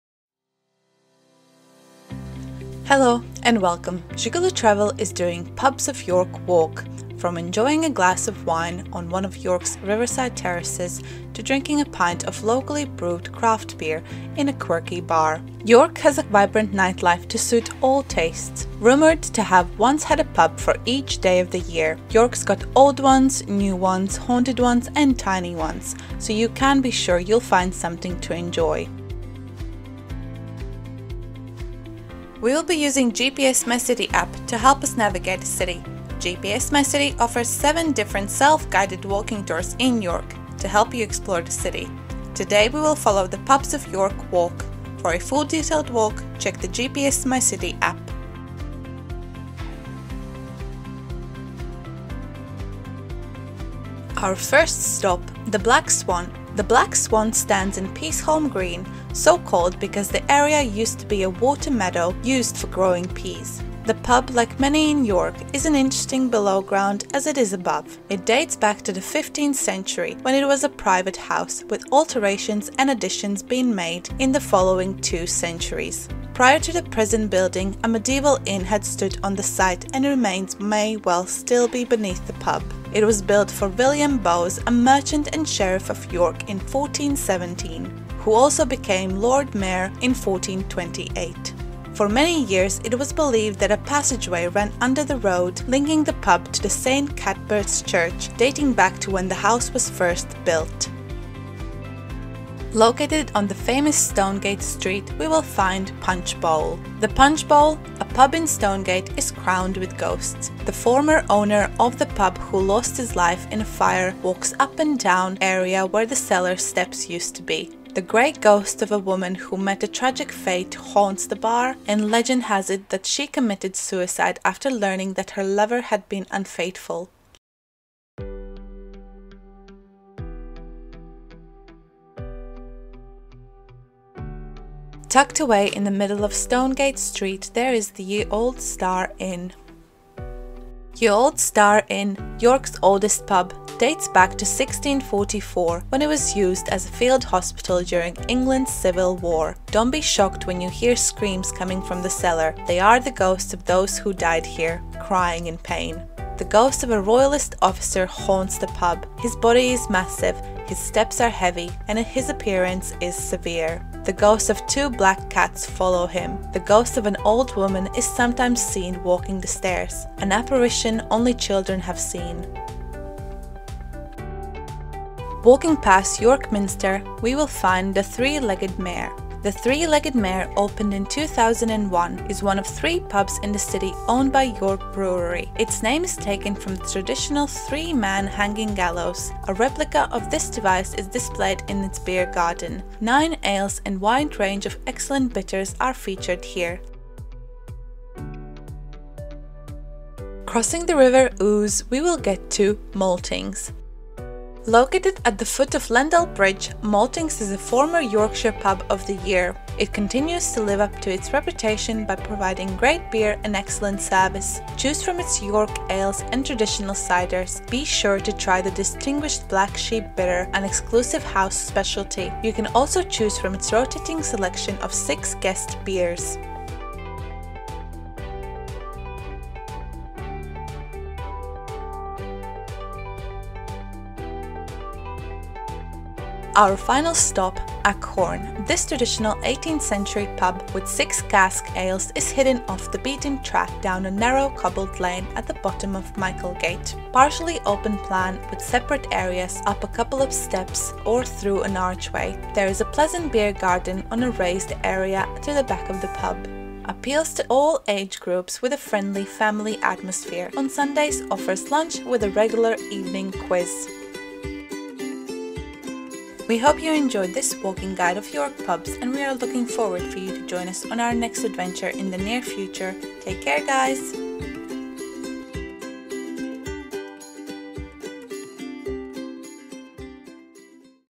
Pubs of York [Travel Guide]
As UK is on track to re-opening some of the services starting April 12, including BEER gardens we decided to revisit some of the places we explored last October in York! We only covered 7 pubs in this video but there are many more for you to explore 😀👍

Pubs of York [Travel Guide] was done with the help of GPSmycity App ⤵️

Pubs of York route and description in full detail ⤵️

🕑 Timestamps 🕑
0:00
0:55 The Black Swan
1:45 Punch Bowl
2:19 Ye Olde Starre Inne
2:55 The Three-Legged Mare
3:23 The Maltings
3:58 Ackhorne

🎥 GoPro Hero 8 (4K50)